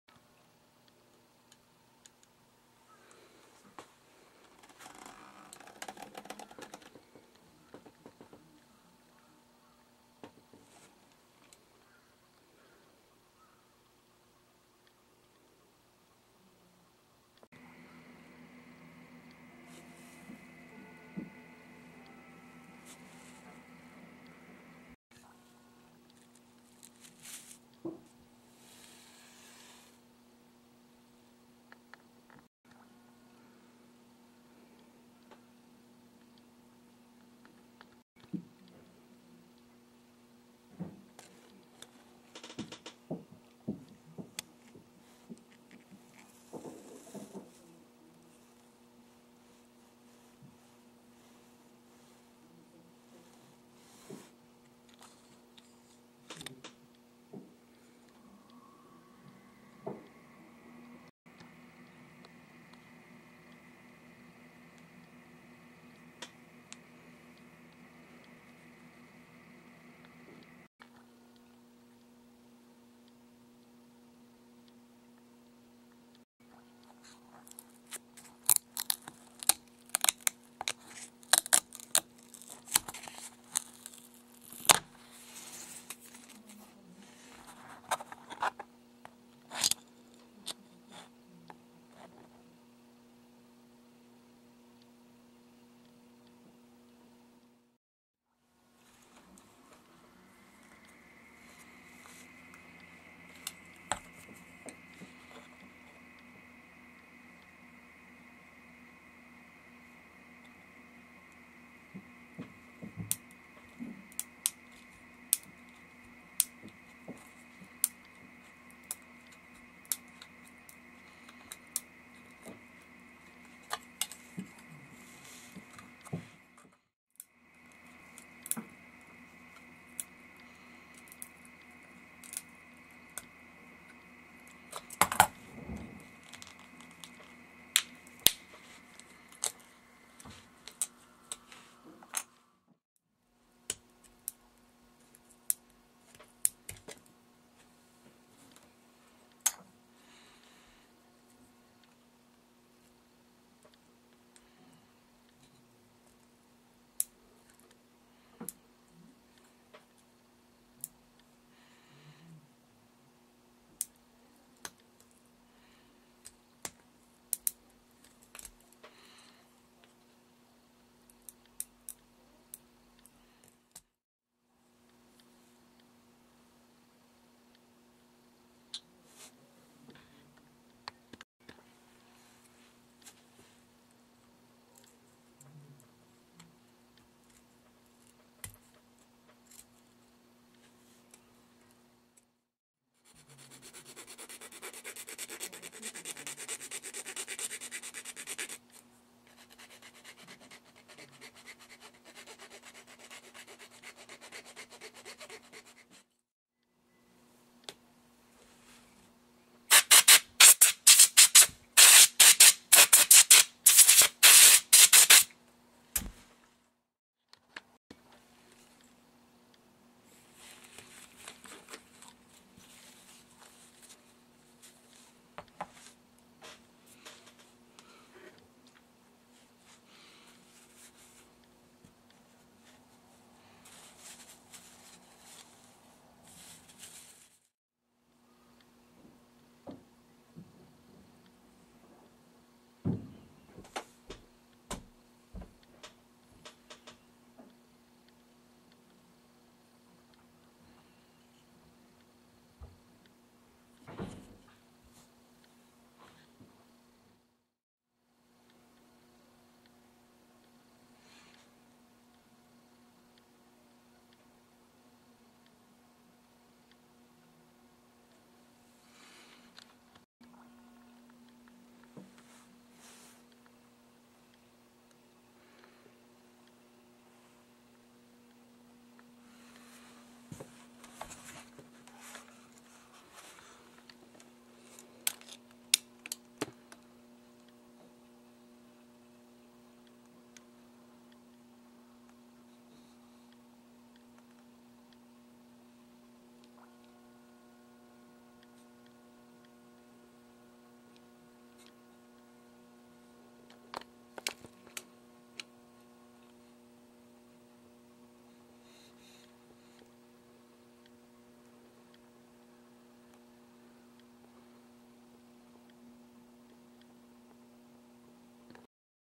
【ダットサイト保護シールド】東京マルイ18禁電動エアガンH&KG36C
ダットサイトシールドをダットサイトの形状に合わせポリカーボネート板で自作。
字幕ＯＮで見てください。